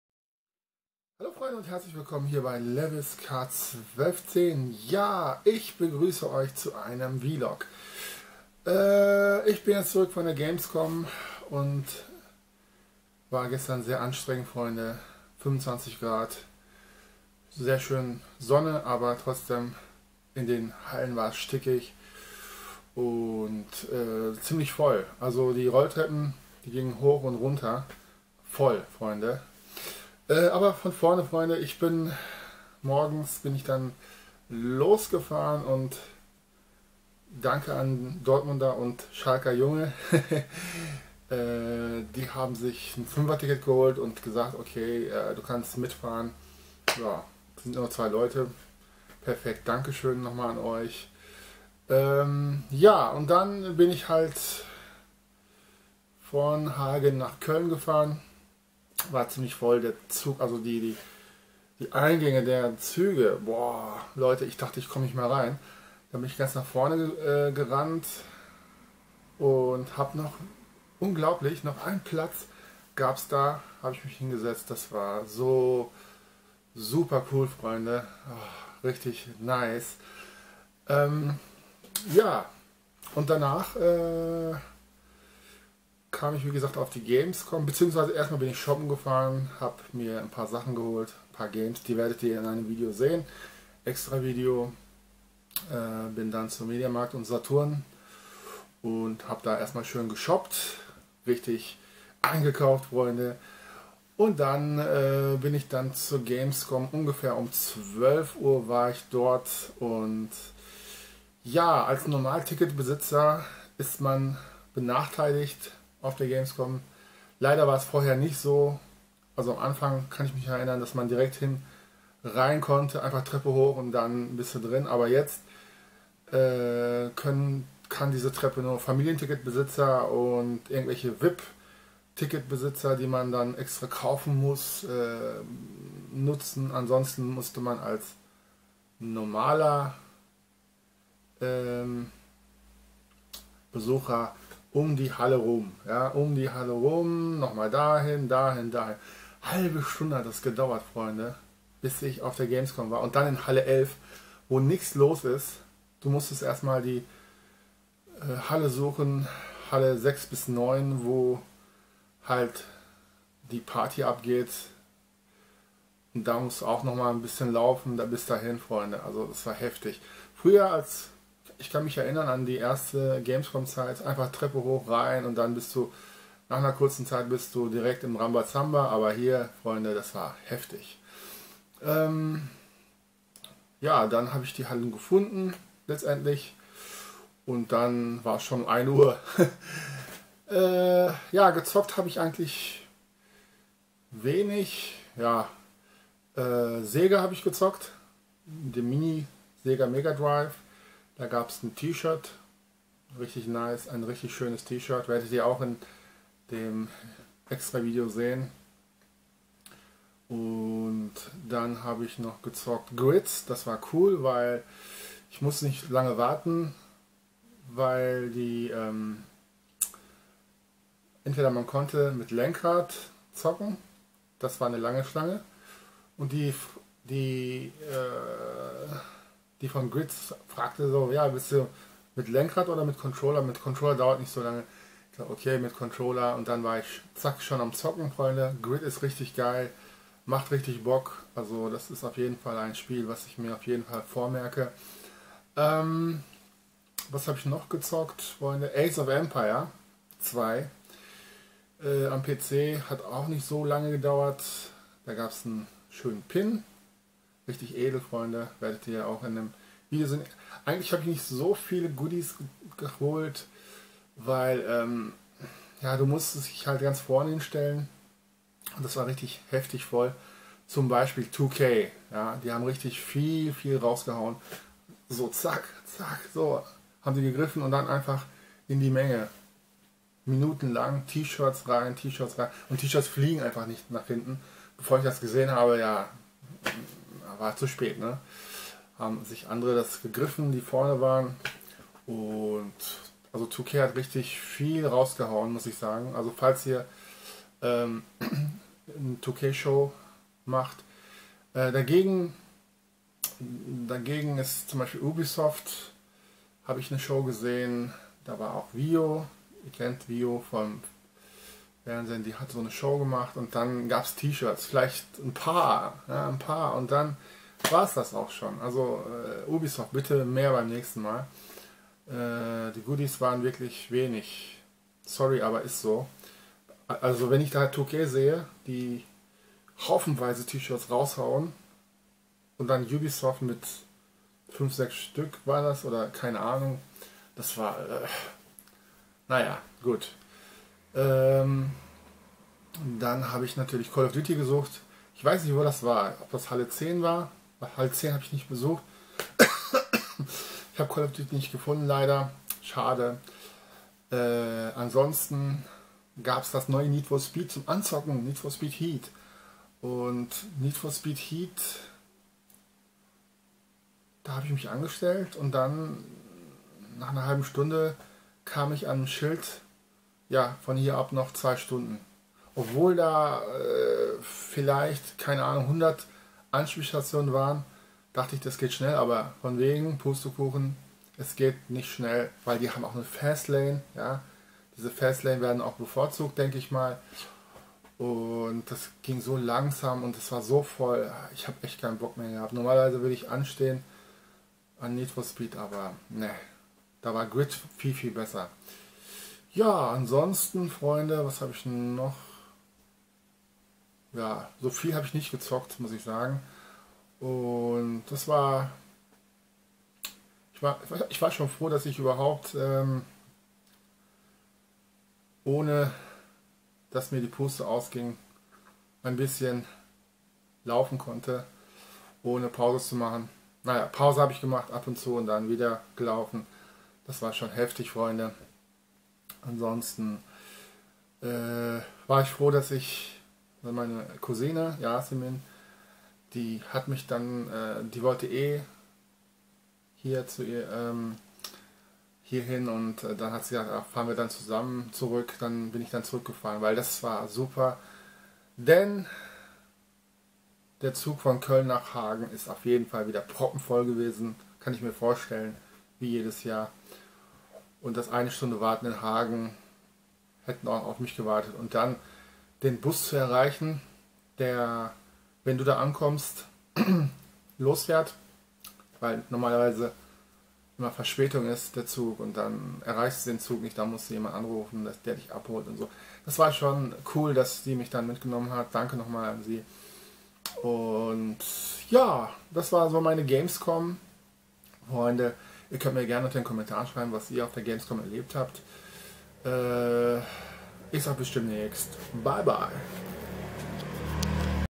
Vlog - Frisch von der Gamescom 2019
So bin zurück und erzähle meine Gamescom 2019 Erfahrungen :)